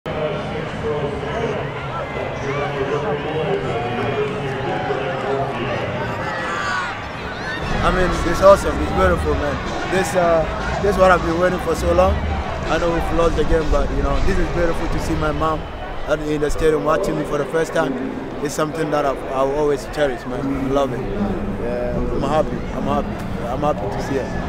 🥹 Lalas Abubakar's mom sees him play for the first time in Colorado
Defender Lalas Abubakar embraces his mom and sister on the field after traveling from Ghana to watch Lalas play in Colorado for the first time.